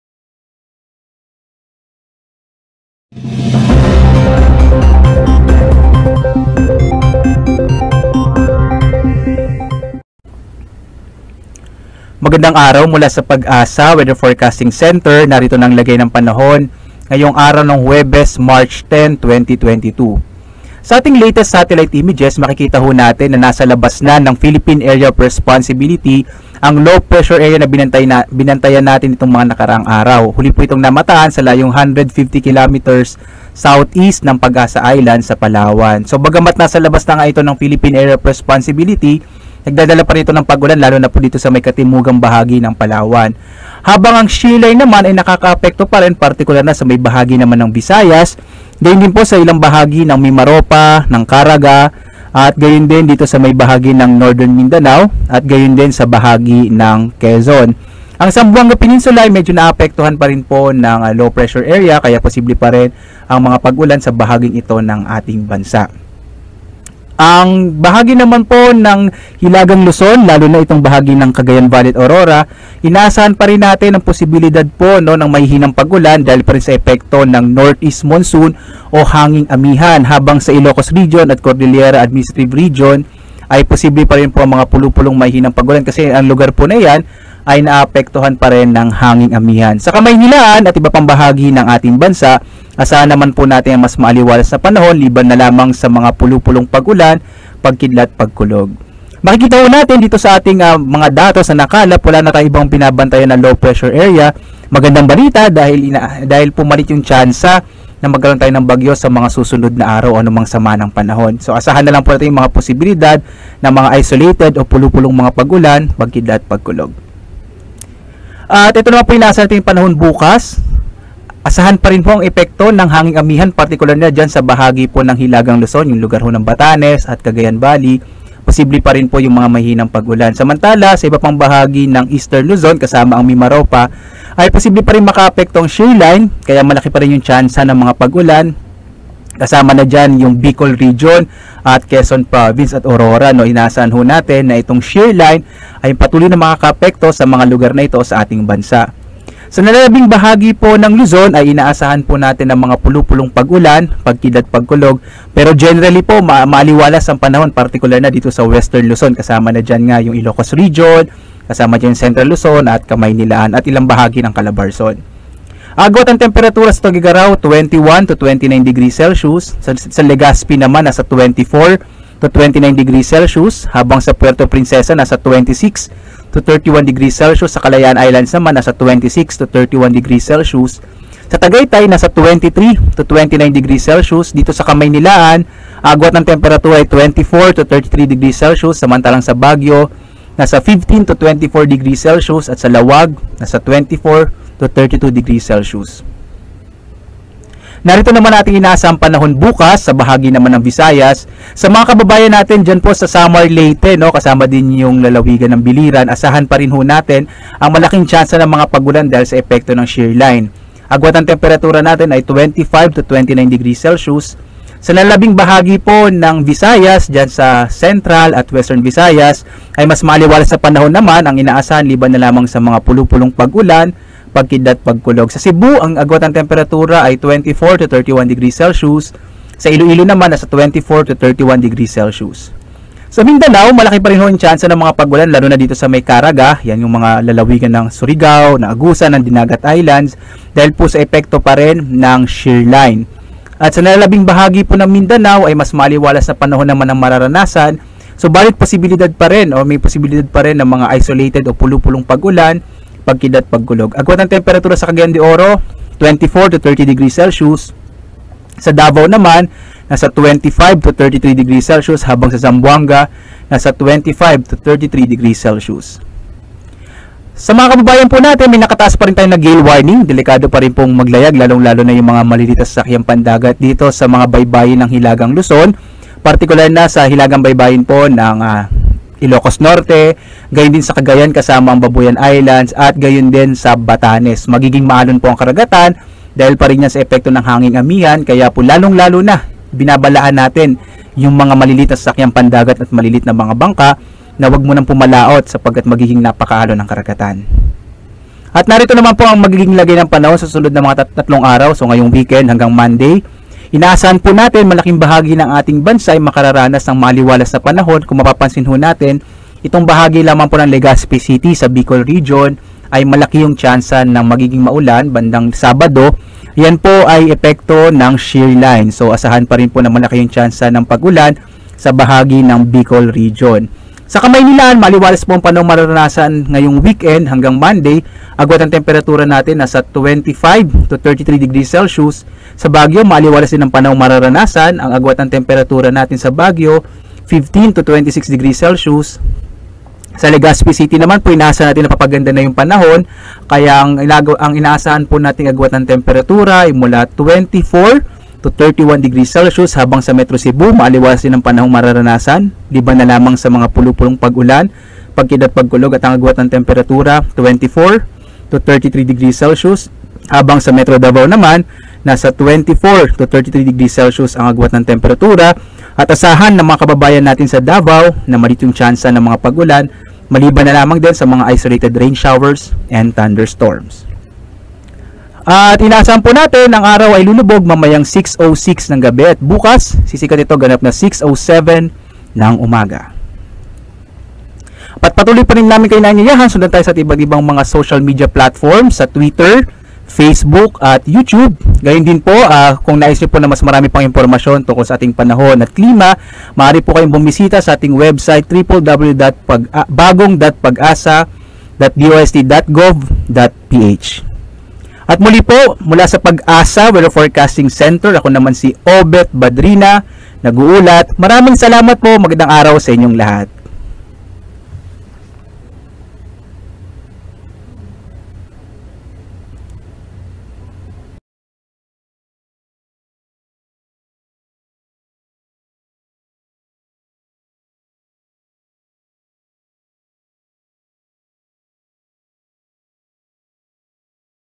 Expect cloudy skies, rain in parts of PH due to ‘amihan,’ shear line — Pagasa
Overcast skies and rain should be expected over several parts of the country on Friday due to the shear line and northeast monsoon, said the Philippine Atmospheric, Geophysical and Astronomical Services Administration (Pagasa). | Via DOST-PAGASA